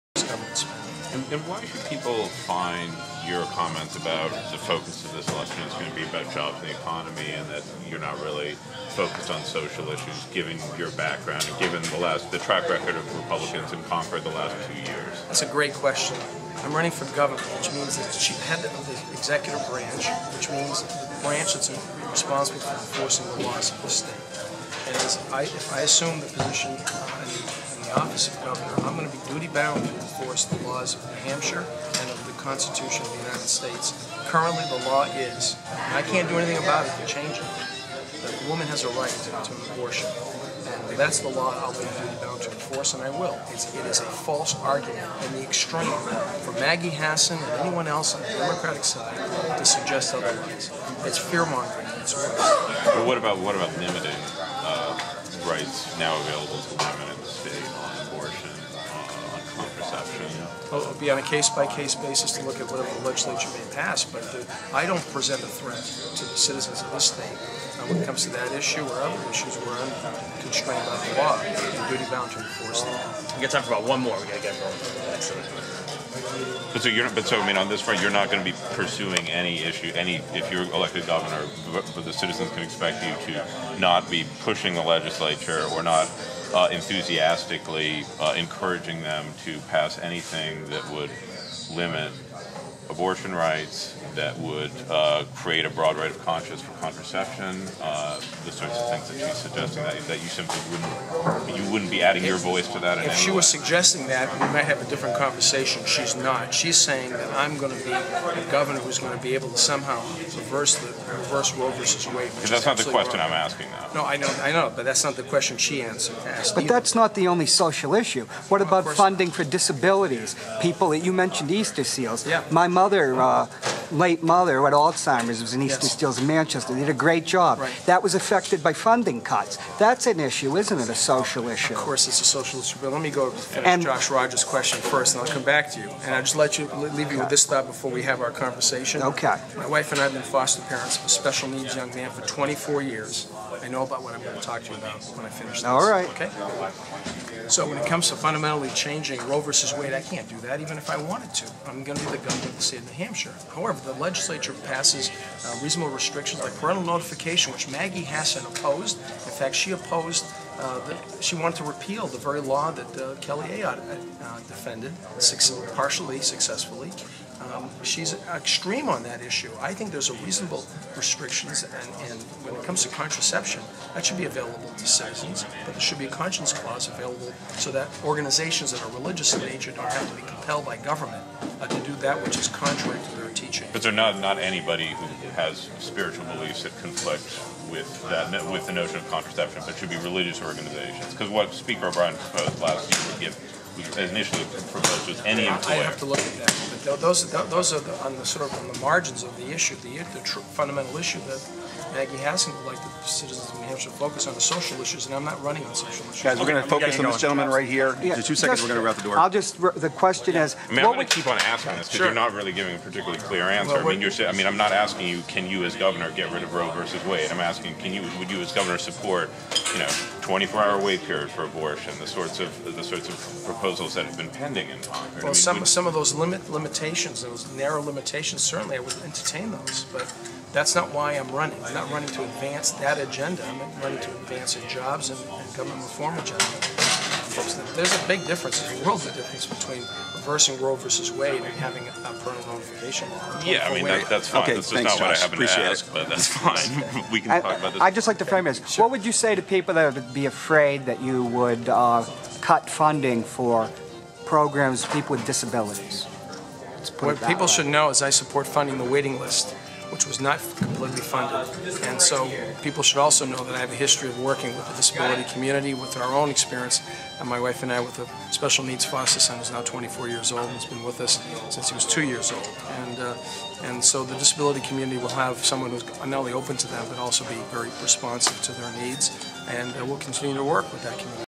Ovide Lamontagne on Abortion and Rights of the Disabled.wmv
Ovide LaMontagne, the Republican gubernatorial candidate, speaks on abortion rights and the rights of the disabled after a debate with Democrat Maggie Hassan at the New Hampshire Institute of Politics (September 19, 2012).